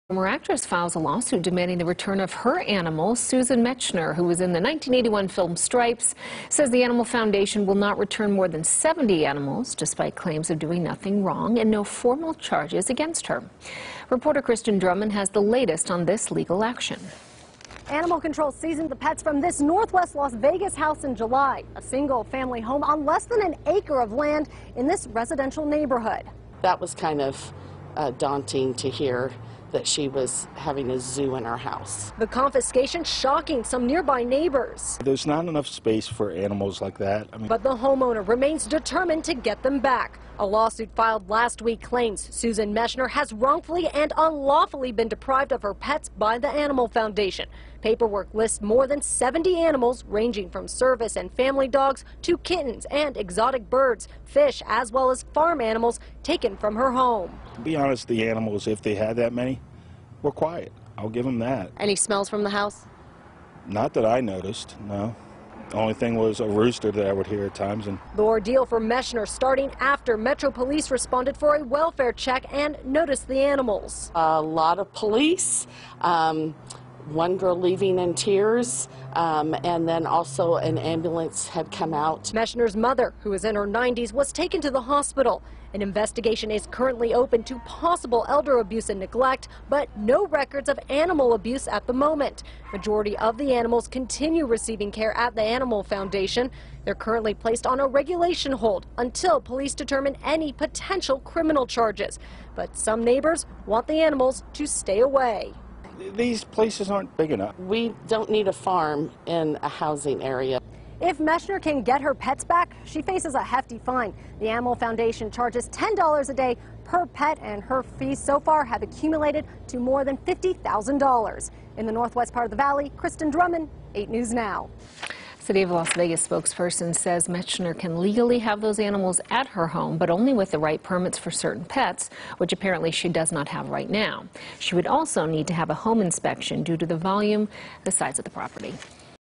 Fmr. actress files lawsuit against Animal Foundation claiming shelter won’t return 70+ animals
A former Hollywood actress filed a lawsuit against the Animal Foundation.  Susan Mechsner, who was in the 1981 film Stripes, says the shelter won't return more than 70 animals despite here claims that she did nothing wrong. She also said no formal charges were filed against her.